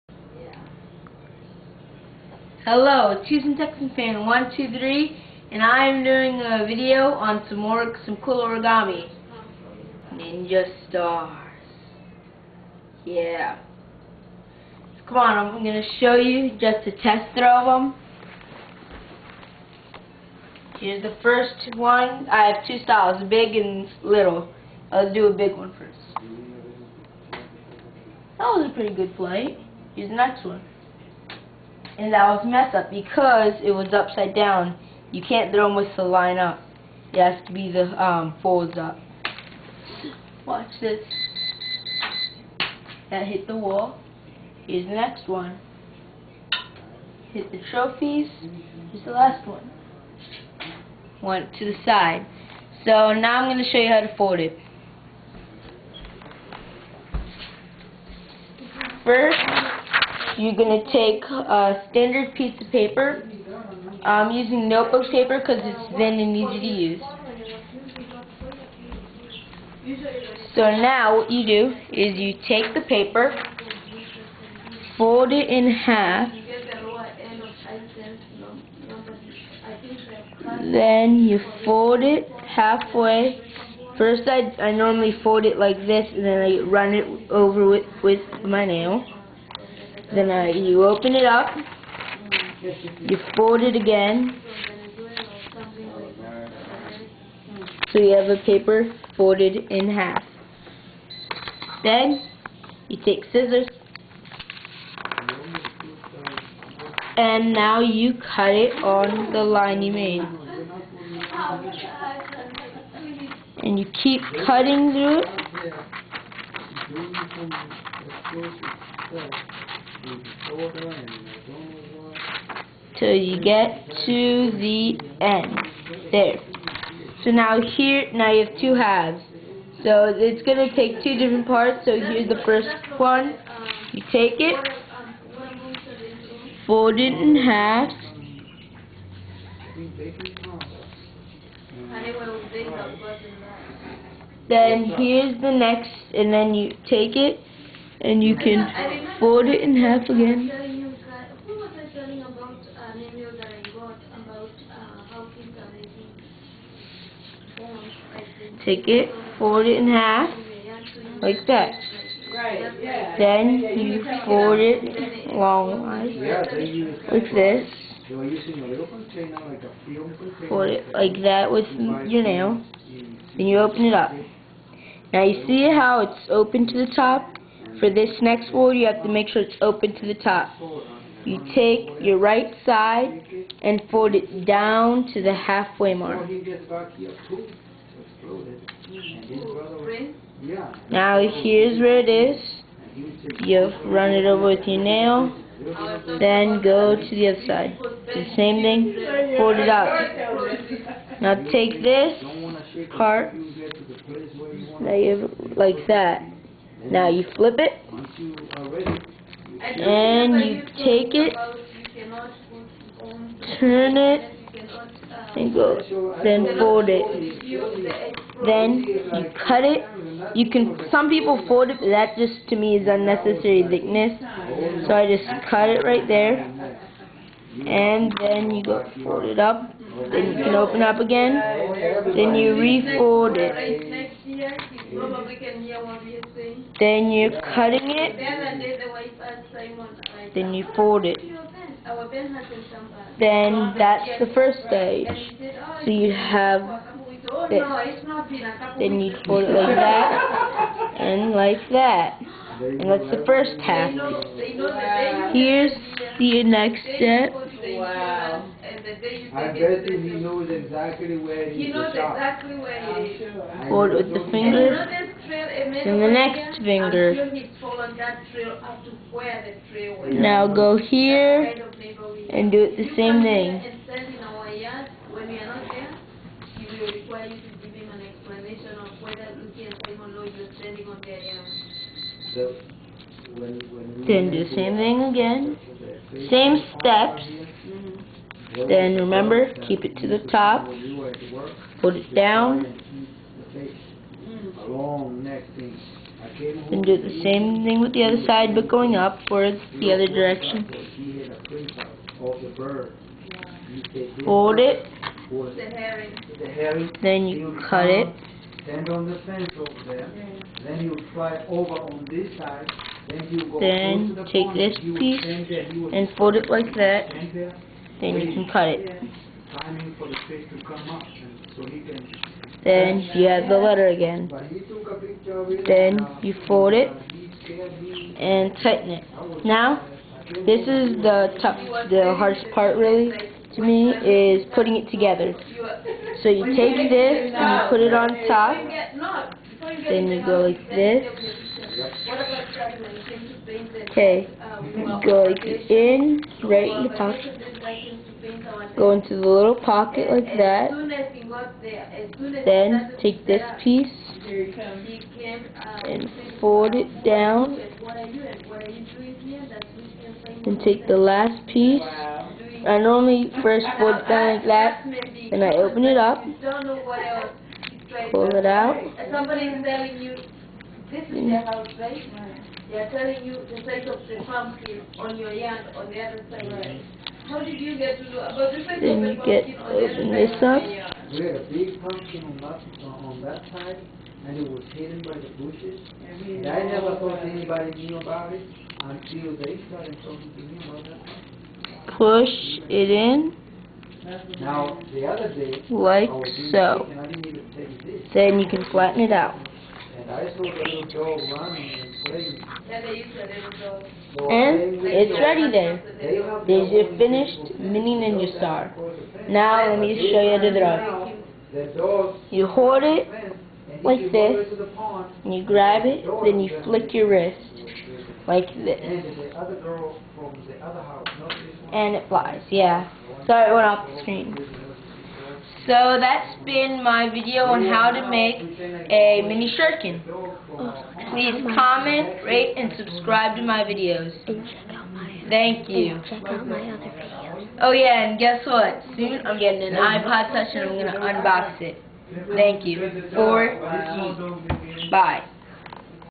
How to an origami ninjastar
This is a video on how to make a mini ninja star.